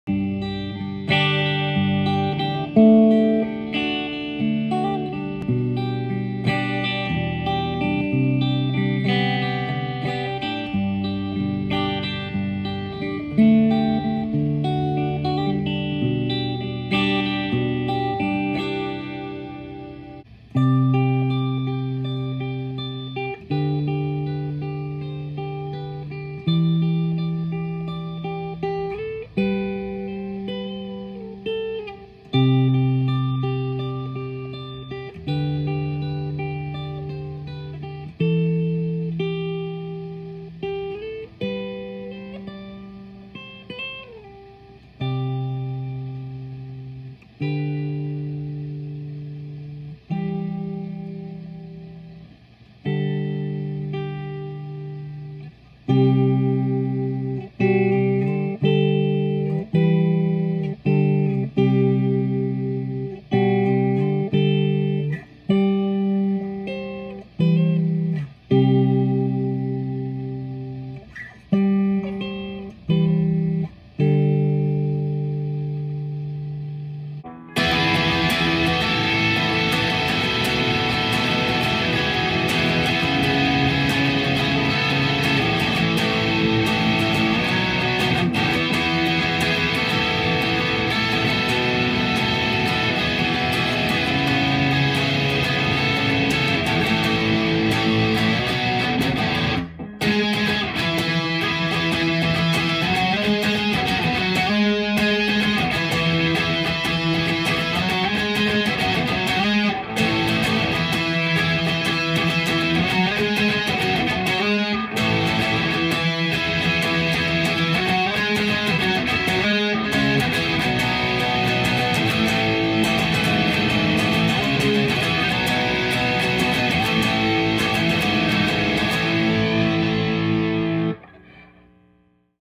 Mesa Boogie F100 Comb Amp With 1967 Epiphone Sorrento
I had to set the master
Volume very low so the camera could get the sound. This amplifier sounds amazing cranked.